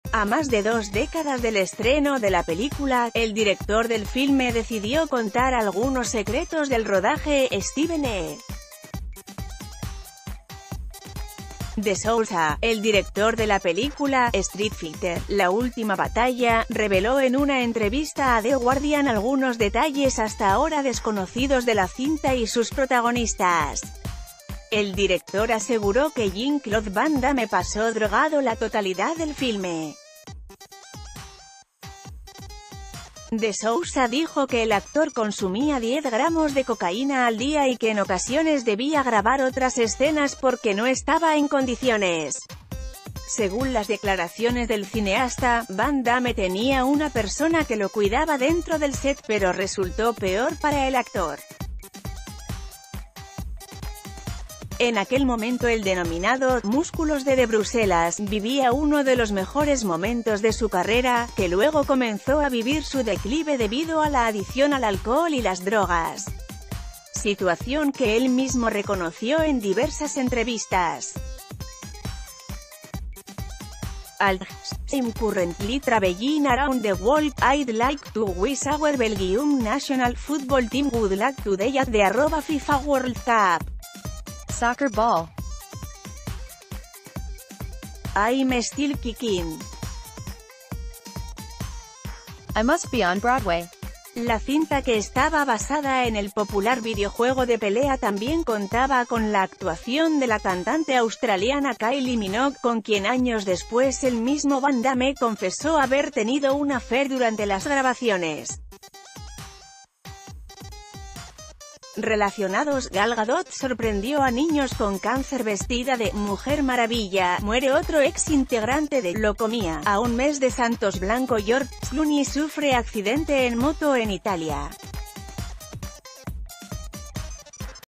Director afirma que Van Damme filmó "Street Fighter" completamente drogado
Cesar Guzman